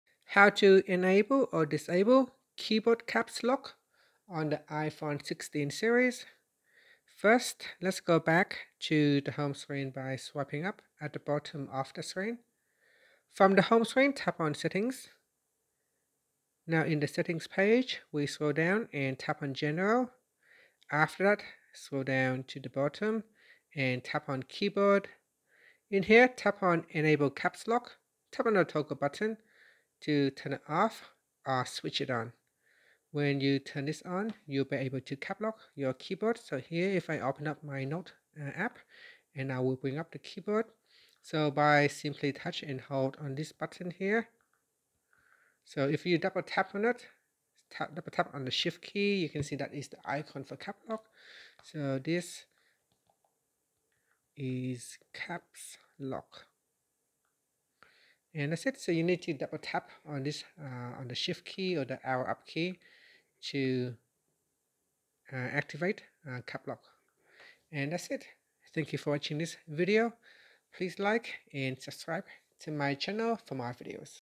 iPhone 16/16 Pro Max: How to Enable/Disable Keyboard Caps Lock
In this video, I will show you how to enable or disable the Caps Lock feature on the keyboard of your iPhone 16 or iPhone 16 Pro Max. Caps Lock lets you type in all capital letters with ease, but if you prefer not to use it or want to prevent accidental activation, this guide will walk you through the process. Adjust your keyboard settings quickly and customize your typing experience.